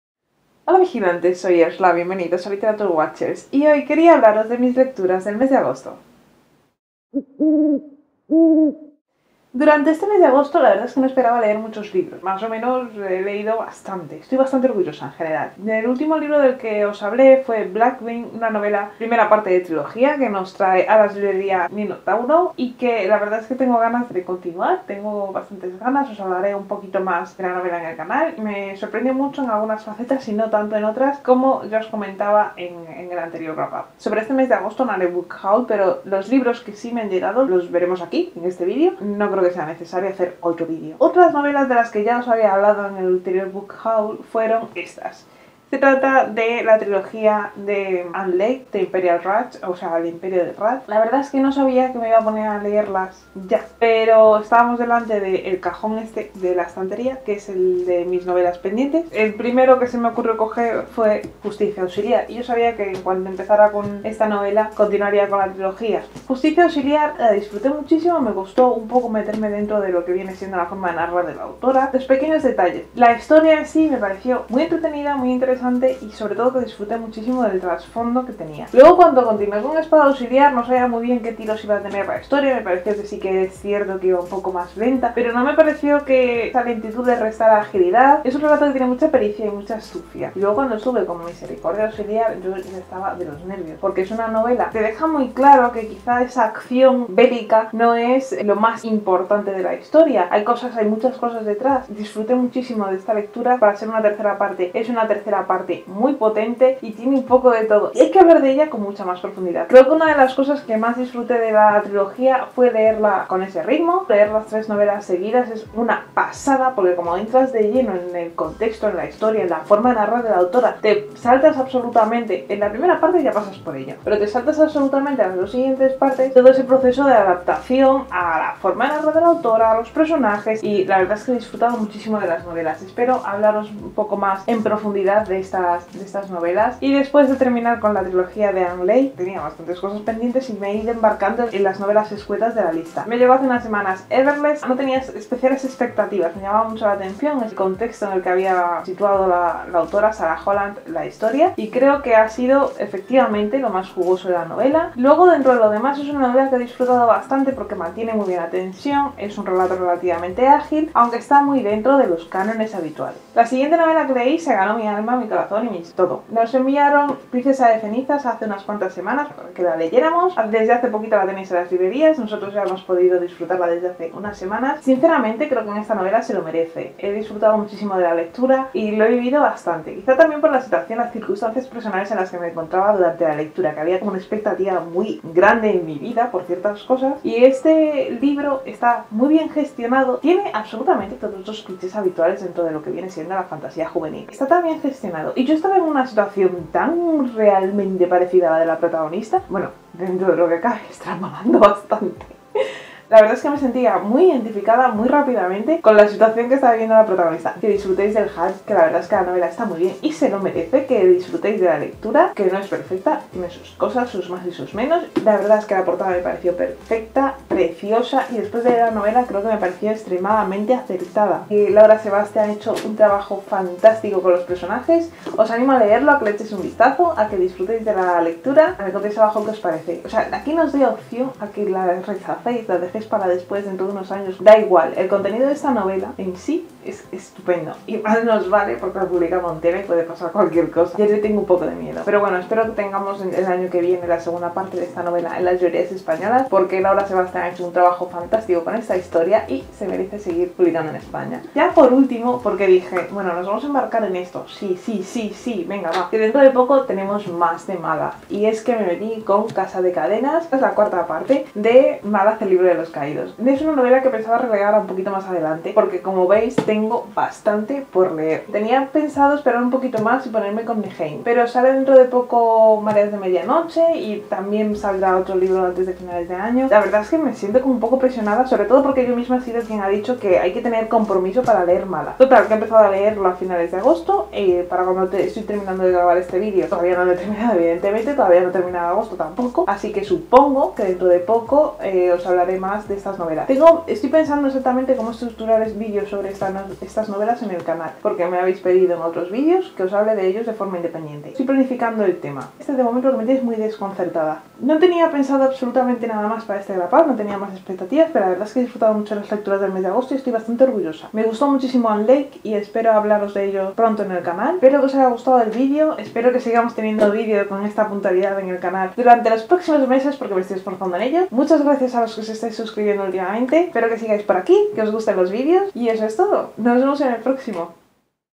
Leído en Agosto| Wrap Up 2018| Ann Leckie/Everless /Princesa de cenizas
Hola vigilante,
Este es el último wrap up del verano, ya se acerca el otoño, nos despedimos de agosto con buenas novelas como la trilogía de Ann Leckie, Everless o la aclamada novedad Princesa de cenizas. Disfrutad del vídeo como yo disfruté de las lecturas compartid vuestras opiniones.

ÁBREME